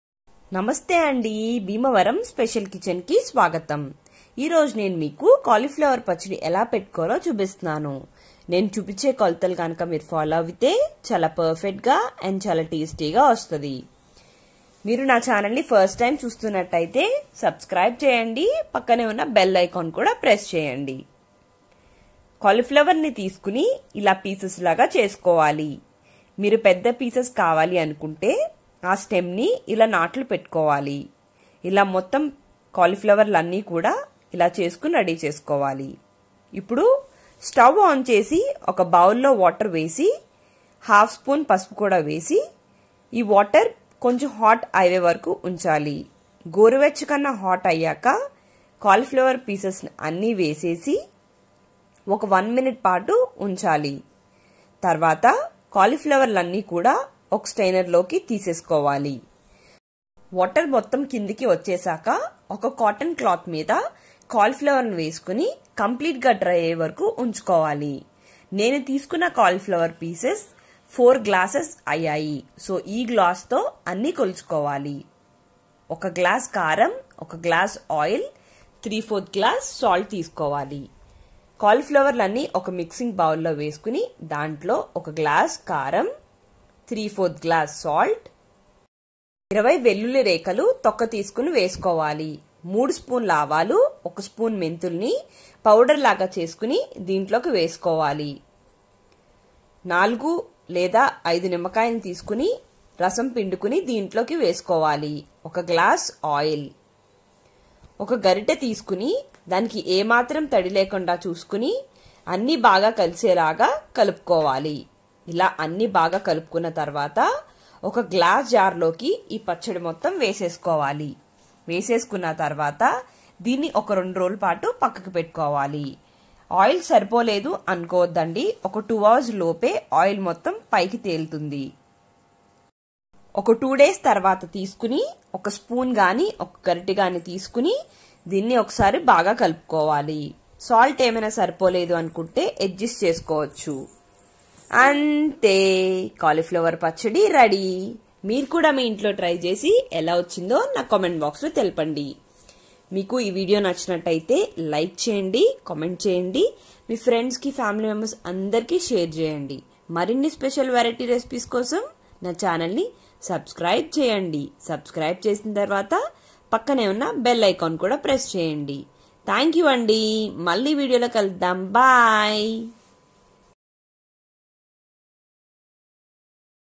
పక్కా కొలతలతో నోరూరించే కాలీఫ్లవర్ ఆవకాయ easy గా తయారీ విధానం || cauliflower pachadi in Telugu
Hi hello welcome to BHIMAVARAM SPECIAL KITCHEN

Today's recipe CAULIFLOWER PACHADI